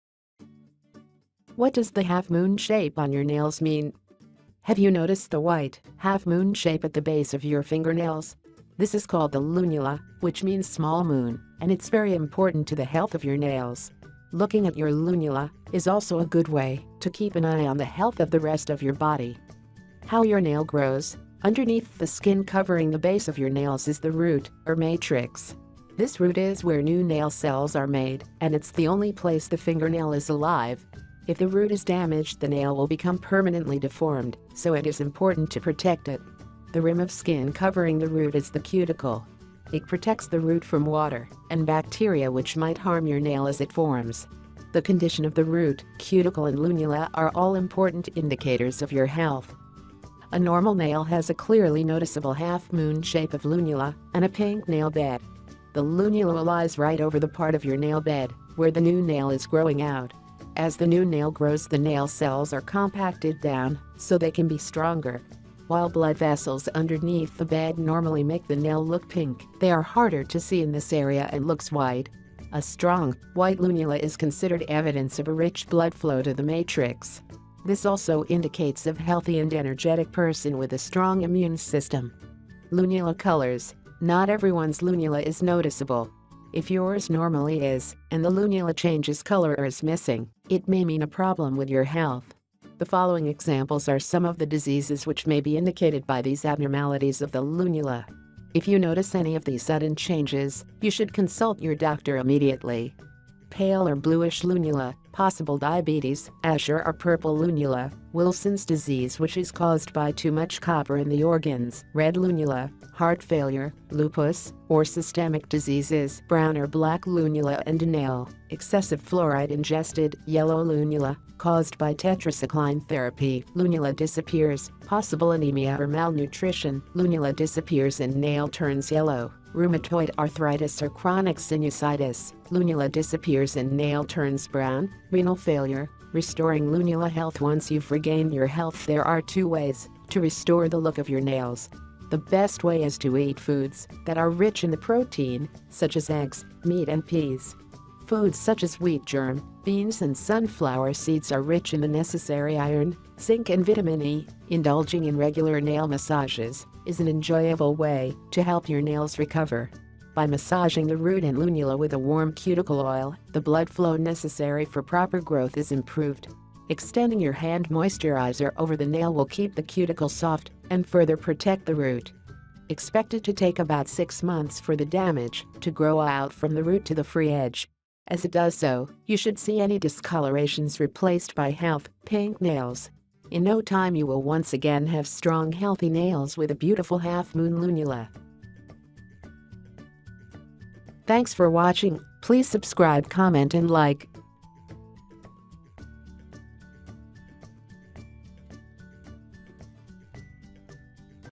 Do you have the half moon shape on your nails! this is what it really means!
Have you noticed the white, half moon shape at the base of your fingernails? This is called the lunula, which means small moon, and it’s very important to the health of your nails. Looking at your lunula is also a good way to keep an eye on the health of the rest of your body.